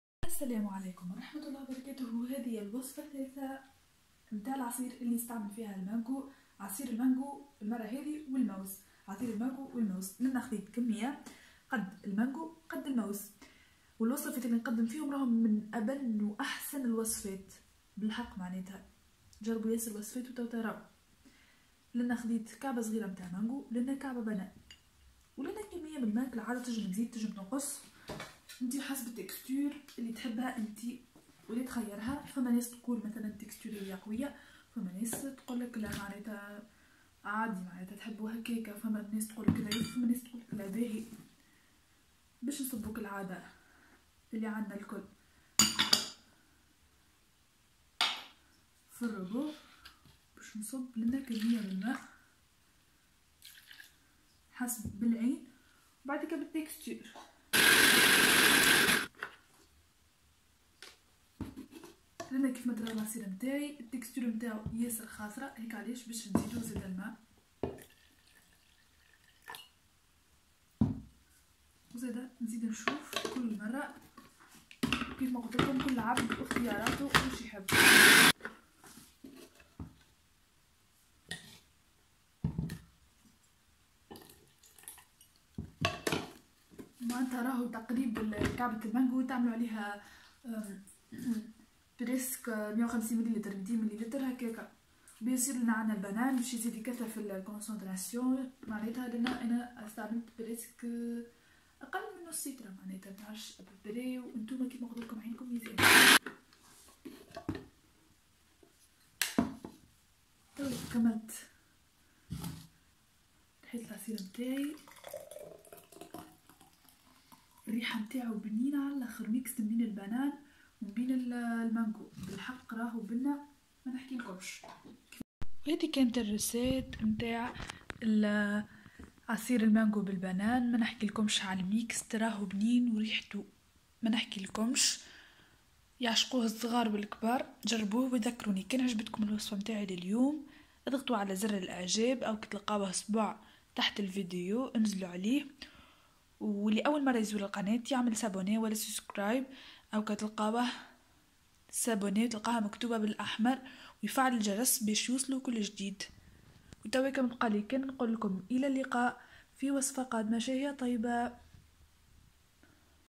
عصير المانغا و الموز ألذ عصير بدون سكر مضاف بدون مصبرات عصير المانغو
عصير المانغا و الموز الطبيعي ألذ عصير موز و منغا عصير المانغو بدون سكر مضاف